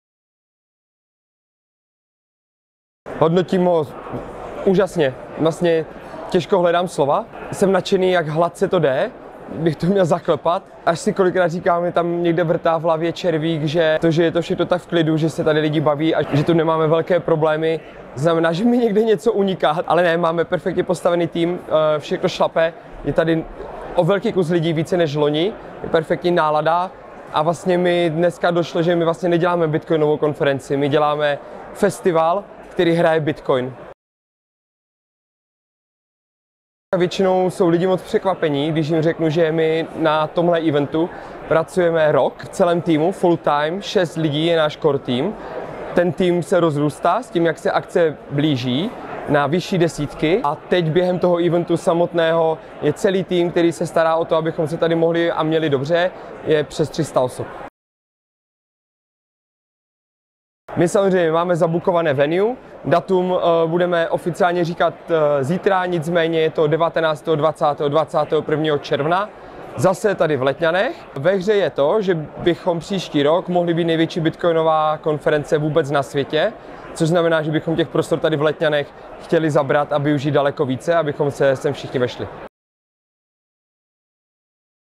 Konference BTC Prague překonala očekávání, příští rok cílí na největší bitcoinovou akci na světě
Bitcoineři se někdy mohou cítit sami nebo ztraceni v nekonečném proudu informací a diskuzí. Nejen proto vznikla konference BTC Prague - největší bitcoinová konference v Evropě, která během již druhém ročníku 13.-15.června nabídla účastníkům prohloubení znalostí o Bitcoinu, rozšíření sítě kontaktů i transformativní zážitky. Nejsme jen svědky budoucnosti bitcoinu, my ji zde utváříme, říkají organizátoři konference.

Jak druhý ročník probíhal očima spoluzakladatele BTC Prague Martina Kuchaře a jaké má konference ambice pro příští rok? Nejen to se dozvíte v následujícím video rozhovoru!